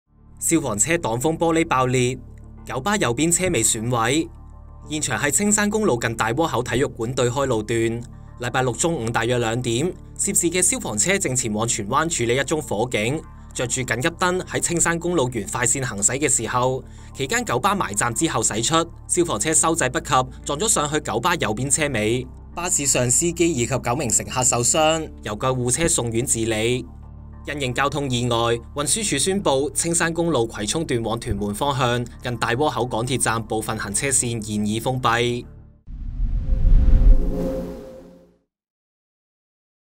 大窩口青山公路消防車與巴士相撞 至少10人傷︳星島頭條︳大窩口︳青山公路︳消防車︳巴士︳車禍
周六（6日）午2時許， 一輛消防車沿青山公路往屯門方向行駛，途至大窩口體育館對開時，與一輛九巴相撞，巴士上10人受傷包括車長及9名乘客，由救護車送院治理，消防車擋風玻璃爆裂，警方封鎖現場調查。受事故影響，青山公路葵涌段左二、三線（往屯門方向）暫時封閉。